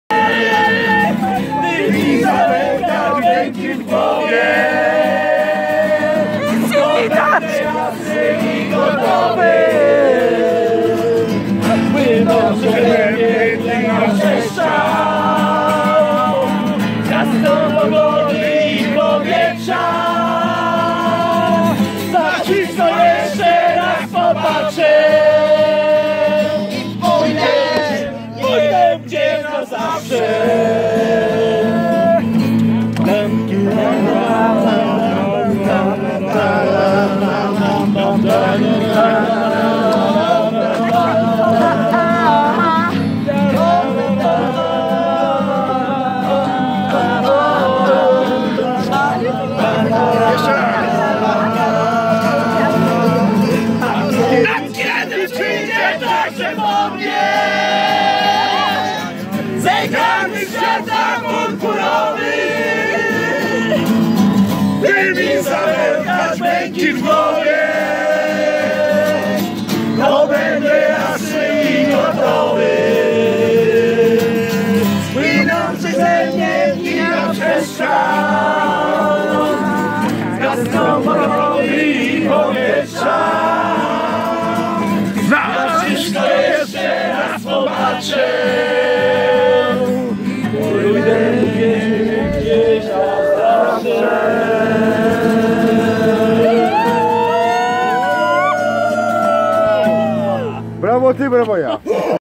Nowy Rok 2018 w Chęcinach Klub Górski Wędrowcy
Tuż po północy były toasty, życzenia, fajerwerki i oczywiście piosenki :)